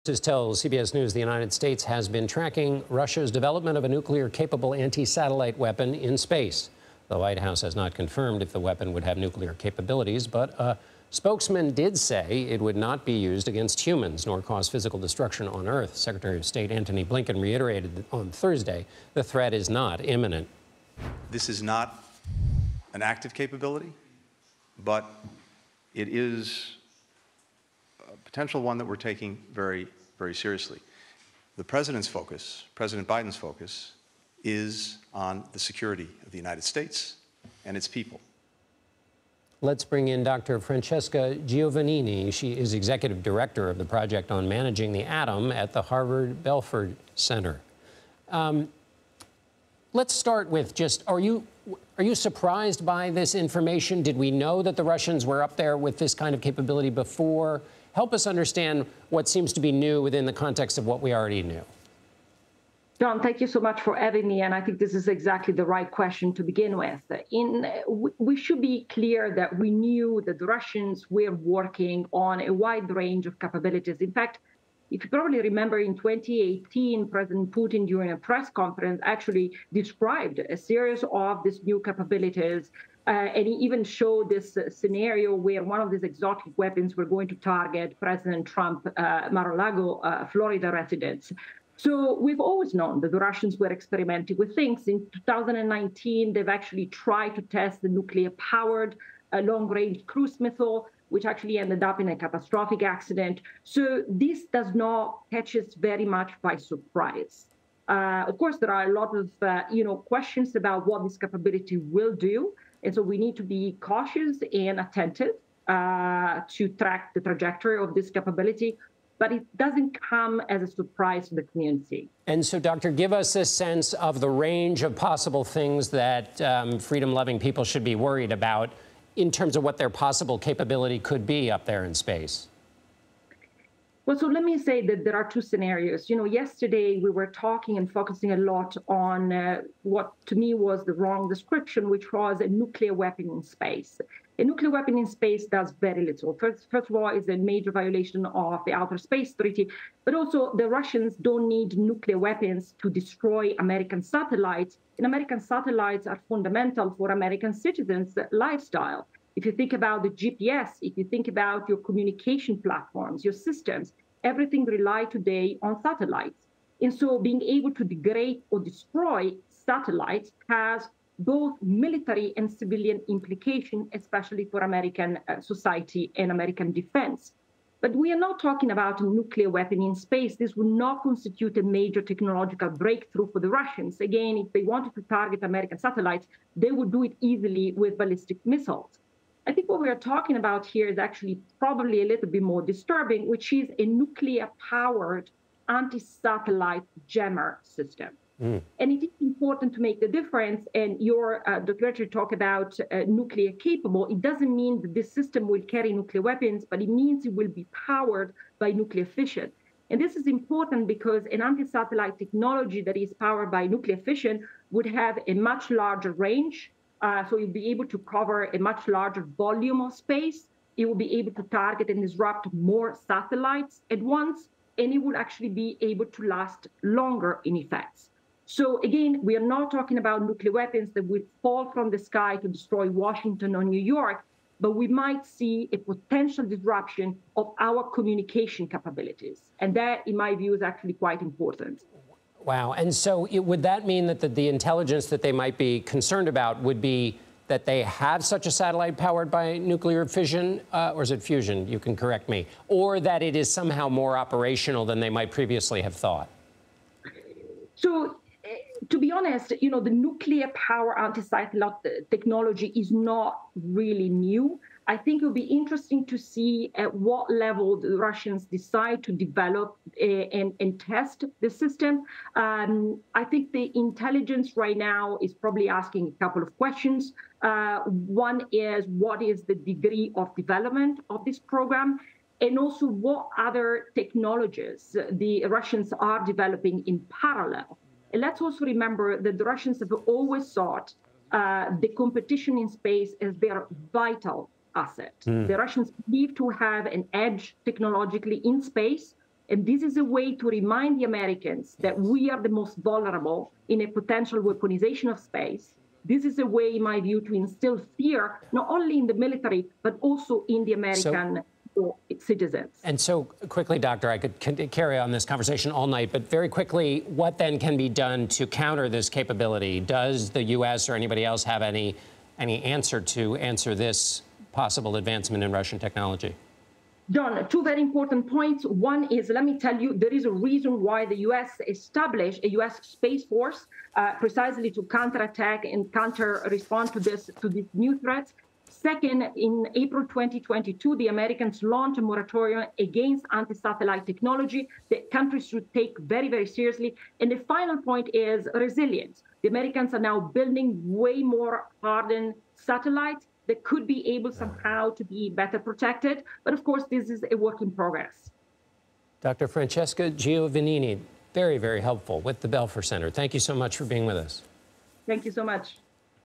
How would Russia's anti-satellite space weapon work?
Sources tell CBS News the U.S. has been tracking Russia's development of a nuclear-capable anti-satellite weapon in space. Dr. Francesca Giovannini, executive director of the Harvard Belfer Center's Project on Managing the Atom, joins CBS News to discuss how the weapon could work in practice.